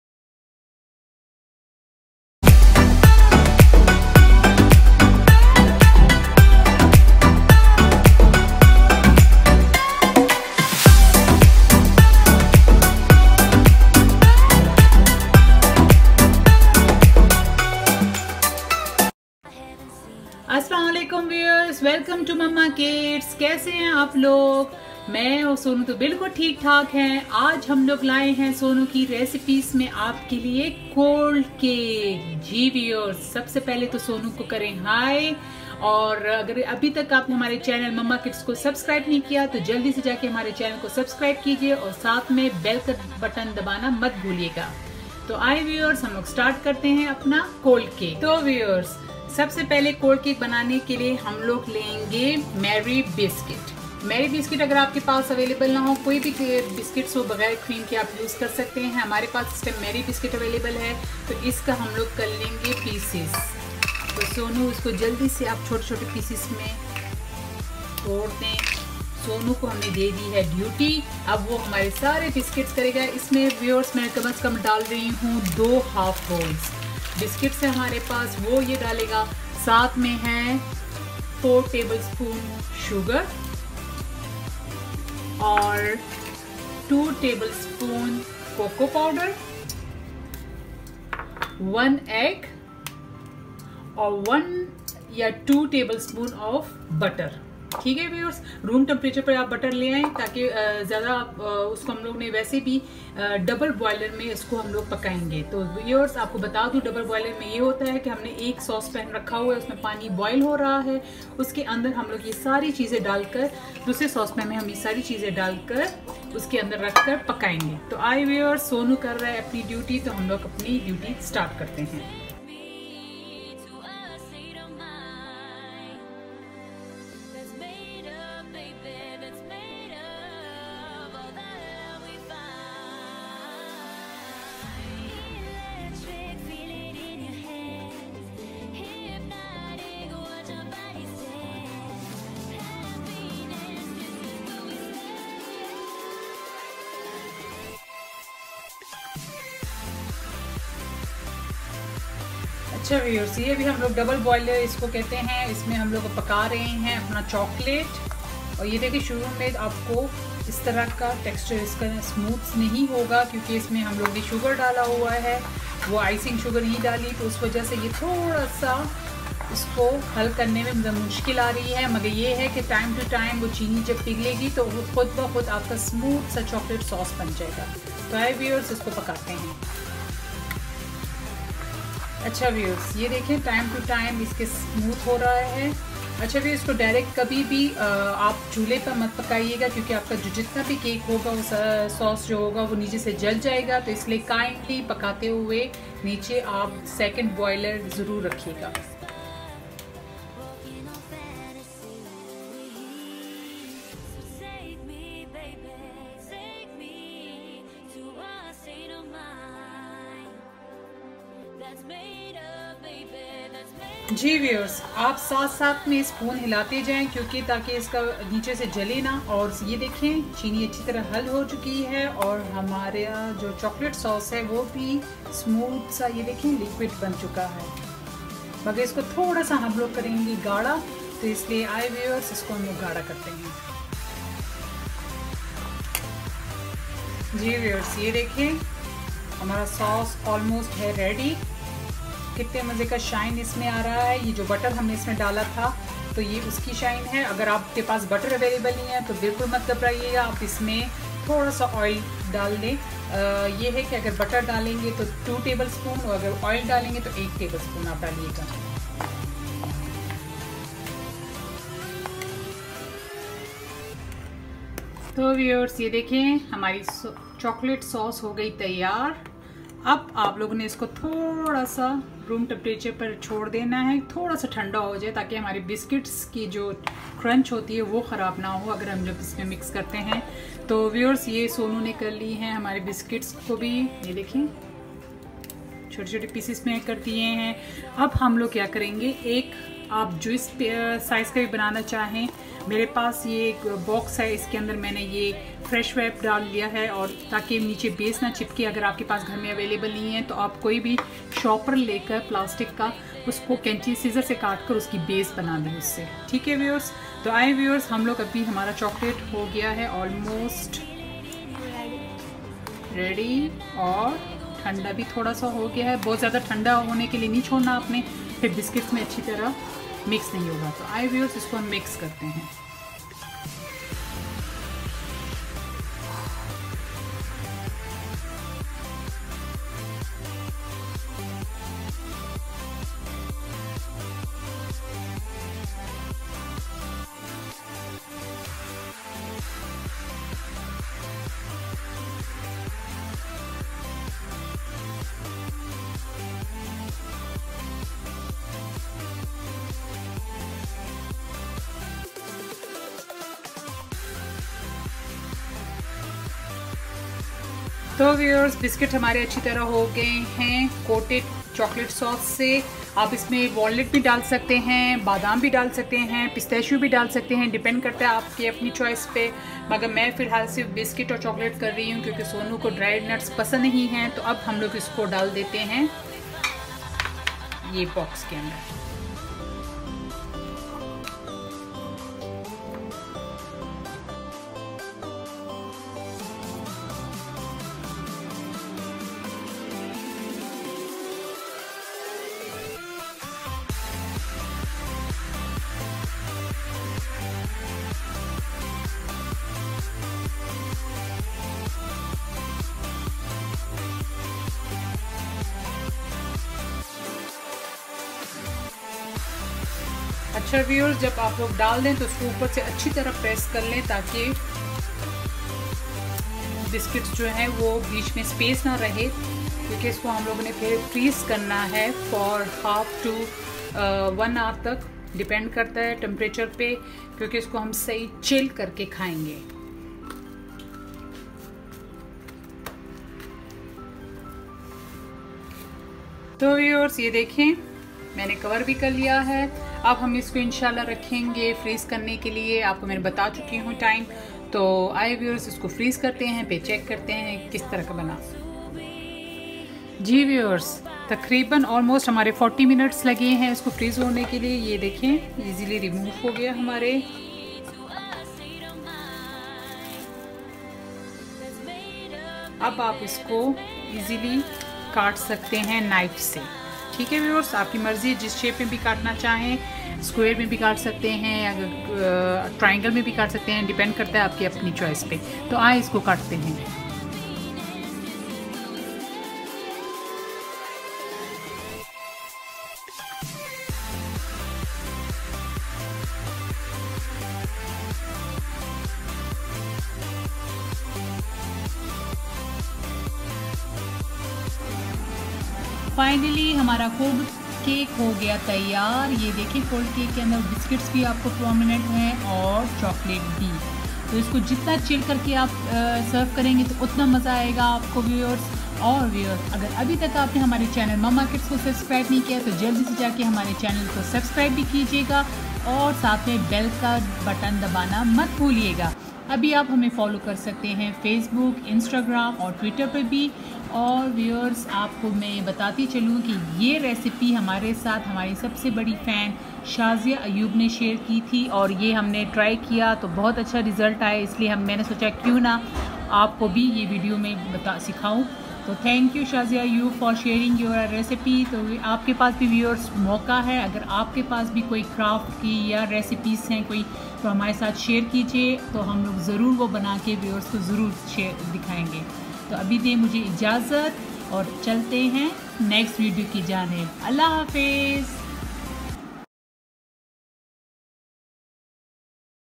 Lazy cold cake| marie biscuit cold cake recipe| chocolate cake| learn with fun
The specialty of this recipe is that this recipe is send by one of our biggest fan SHAZIA AYUB.
Thank you once again and thanks to all my viewers too for sharing their recipes and tips .InshaAllah soon i share it too in my upcoming videos

🍽This recipe is easy and delicious.Ingredients are simple like
Biscuits
Sugar
Coco powder
Egg
Butter or cooking oil

Music 🎶

stags
Marie biscuits cold cake recipe
chocolate cake
in urdu
in hindi
quick recipes
maama kids
Biscuit cake